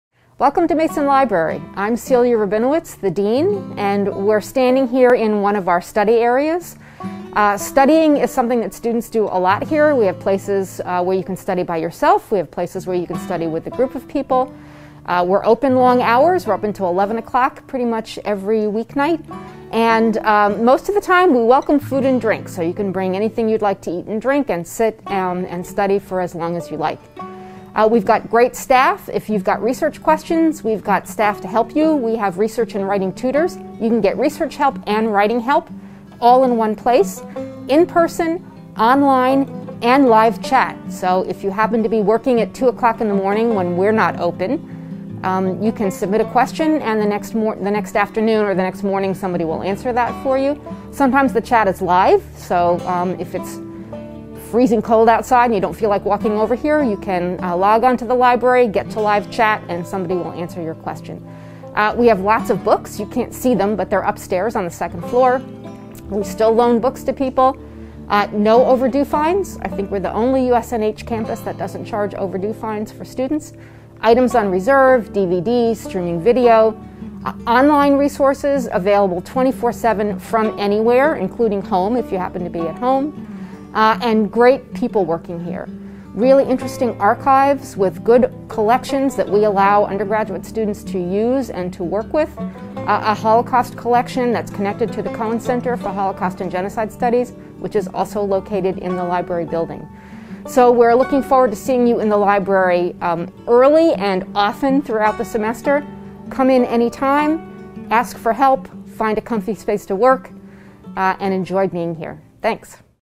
Mason Library at Keene State College
The Wallace E. Mason Library provides you with materials and services in support of the college’s undergraduate and graduate curricula. The library staff provides students with information services and instruction in research methods and the use of library resources. Collections housed in Mason Library include over 280,000 items with subscriptions to more than 65,000 print and electronic journals and newspapers, an extensive selection of audiovisual materials, and the Archives and Special Collections, which are valuable collections that you can use as a student. You also have ready access to the 100,000 volumes available at the Keene Public Library.

FOLLOW Keene State on social media!